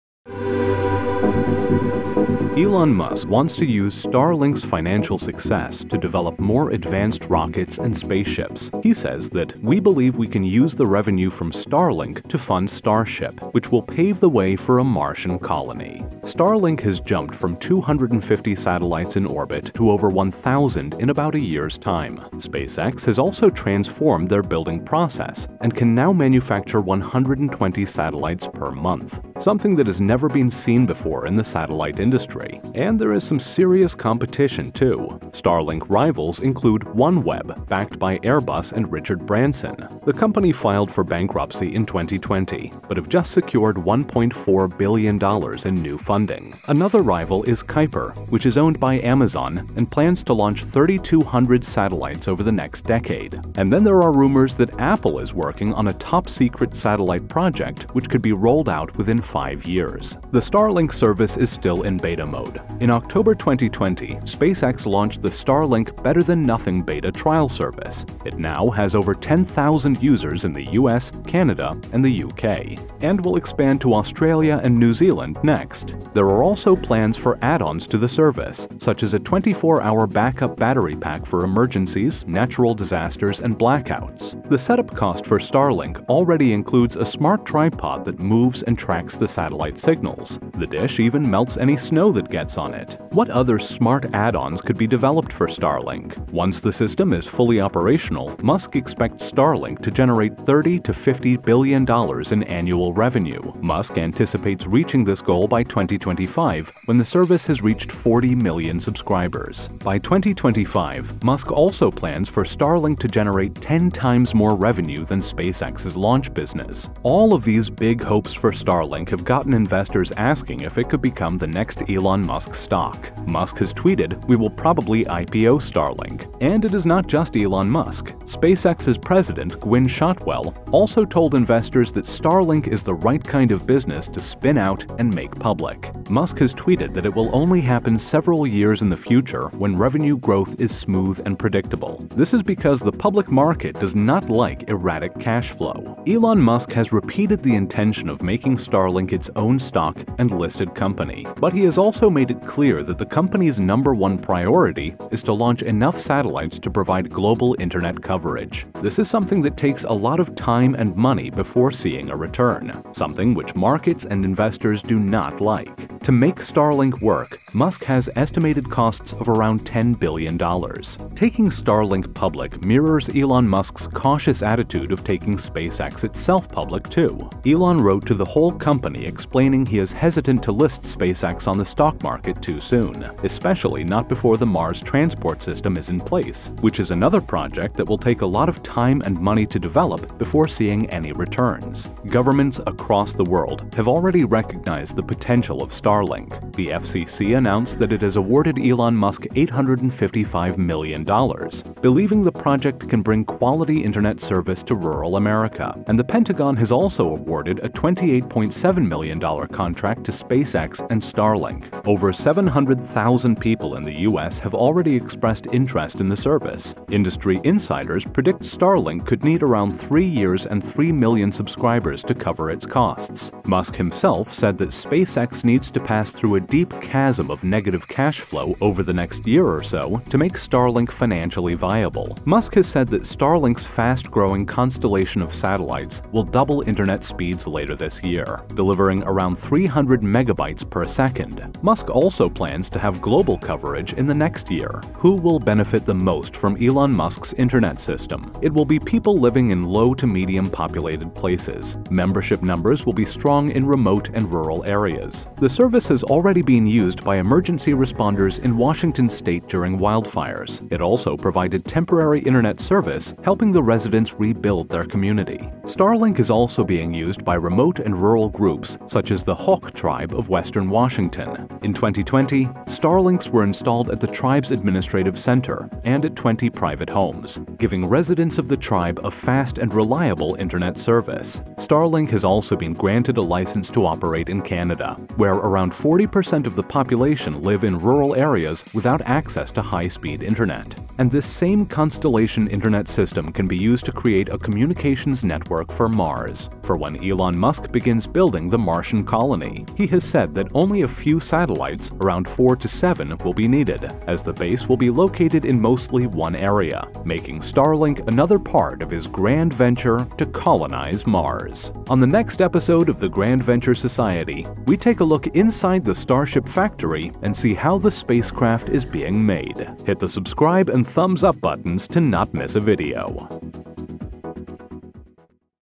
Elon Musk Revealed Starlink Future Plans (Add-Ons, IPO)
Will SpaceX's Starlink project become Elon Musk's next stock listing. He along with the President of SpaceX, Gwynne Shotwell, has said that Starlink is the right kind of business that could be spun out and have an IPO.

But there is a lot of work that needs to be done first to make Starlink's internet service profitable. And Elon won't list Starlink as a public company until things are stable. Just like how he does not want to list SpaceX, until his Mars mission plans are up and running.

Other topics covered in this video include:
• Who's benefiting from the Starlink service today (emergency responders, rural tribes)
• Who else is competing in this internet satellites space (from Amazon to Apple)
• When could we see the Starlink stock price
• Other ways Starlink could make money (add-ons, battery packs)
• Starlink's launch growth and future plans explained